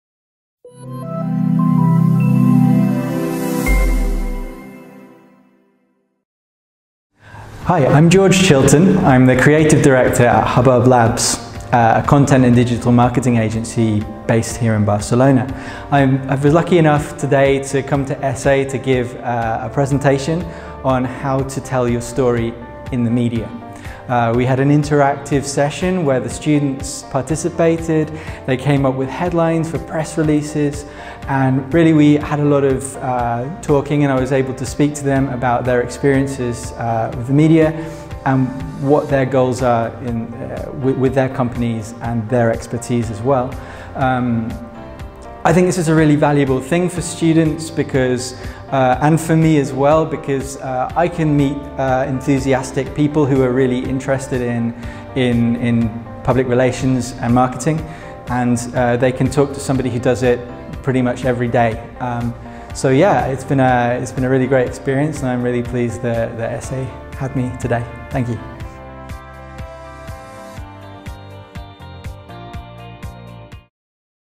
ESEI Guest Speaker: George Chilton, Creative Director at Hubbub Labs
Friday 10th of November, we welcomed George Chilton, Creative Director at Hubbub Labs for a talk on "Storytelling: How to tell your story to the media".